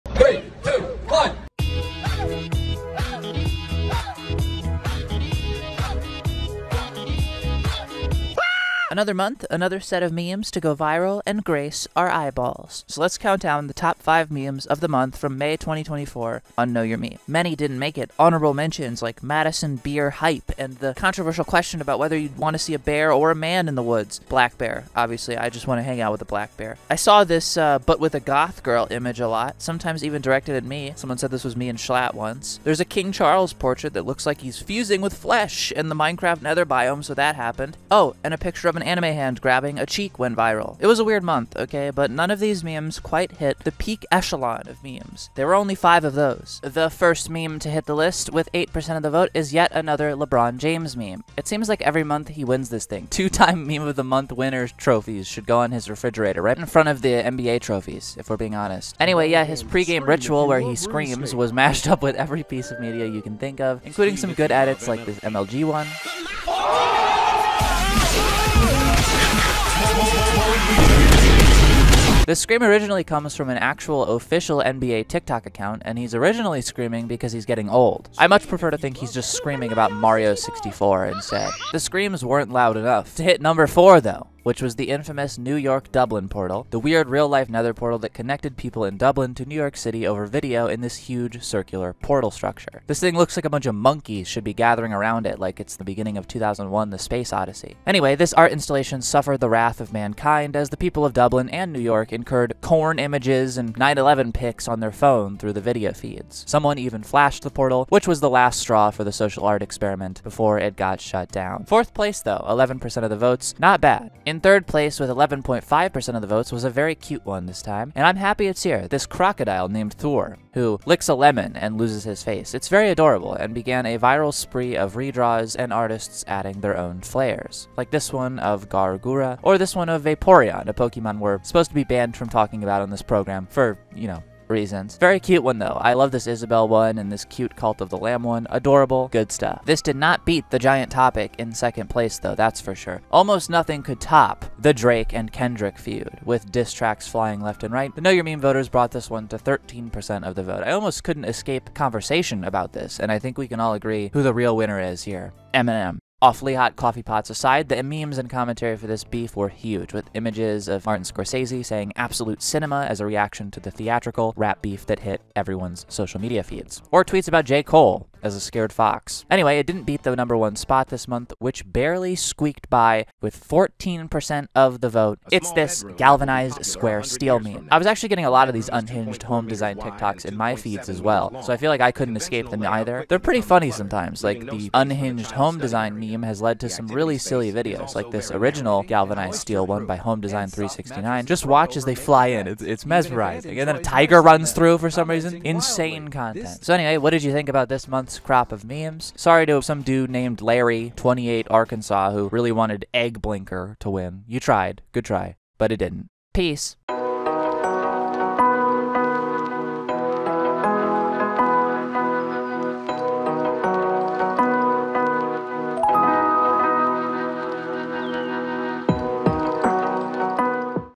May 2024's Meme Of the Month Revealed
@Aztrosist lists off May's top memes as voted on by the users of Know Your Meme. We had another LeBron meme, fascination with the Dublin-New York Portal, no one would shut up about the Drake-Kendrick beef, and we all agreed we'd love to borrow some galvanized square steel beams from out aunt.